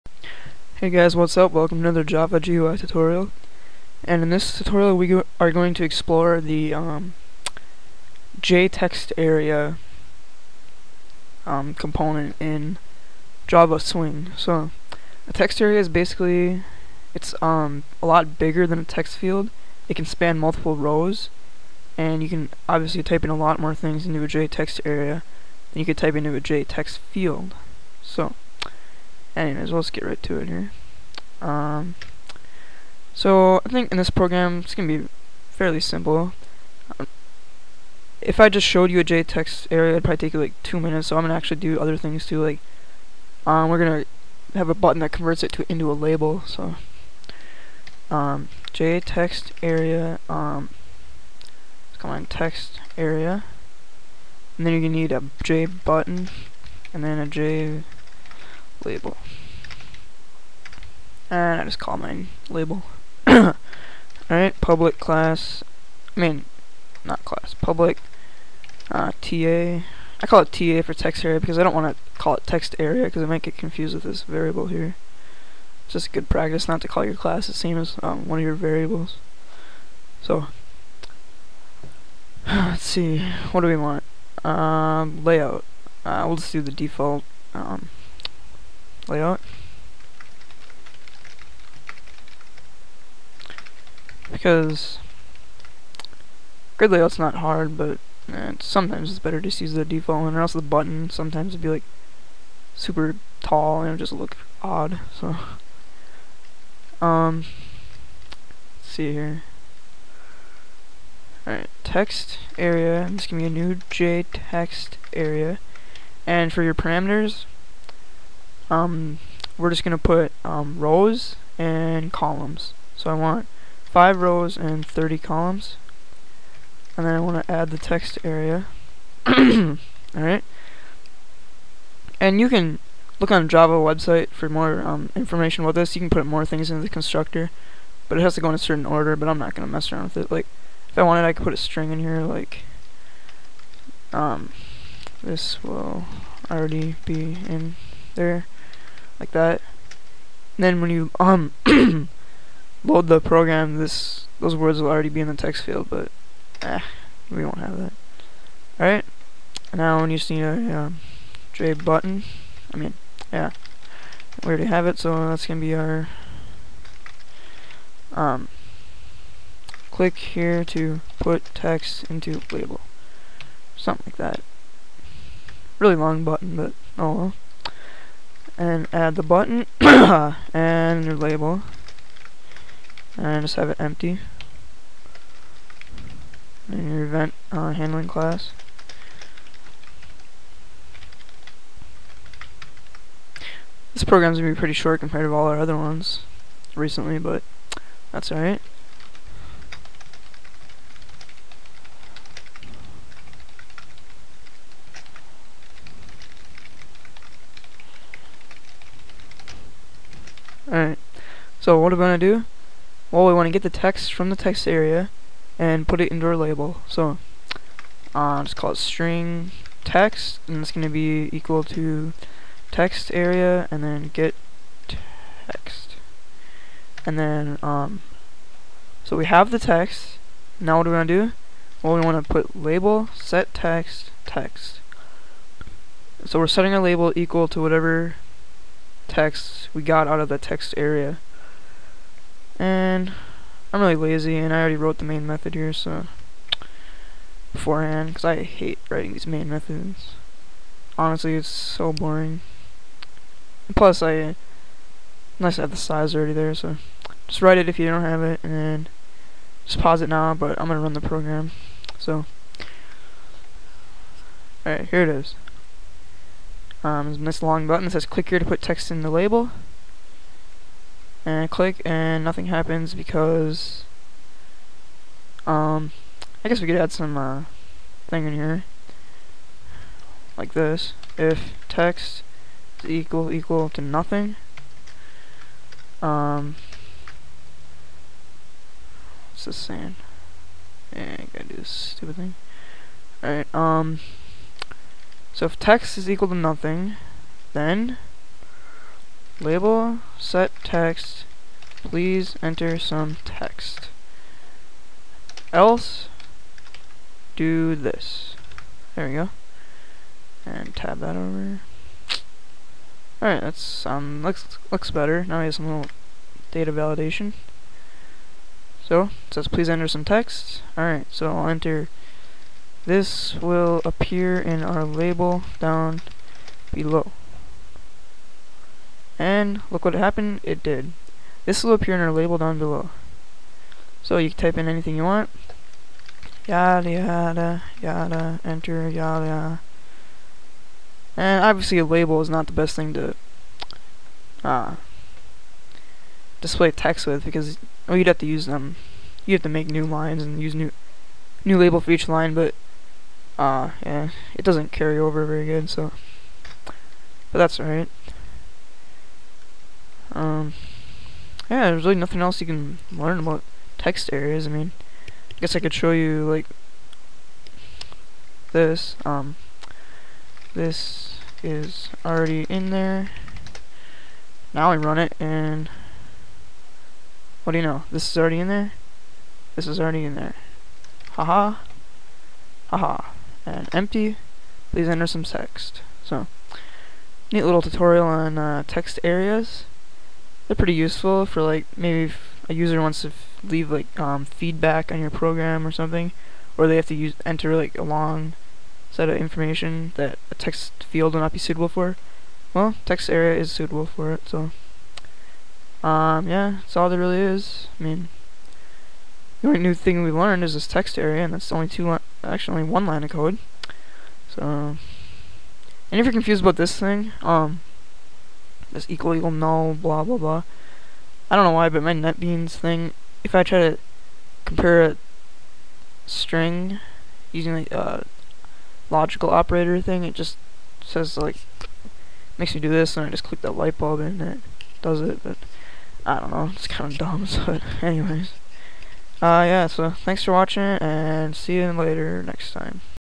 Java GUI Tutorial 28 - JTextArea
Now you will learn what  JTextArea is and how it differs from a textField.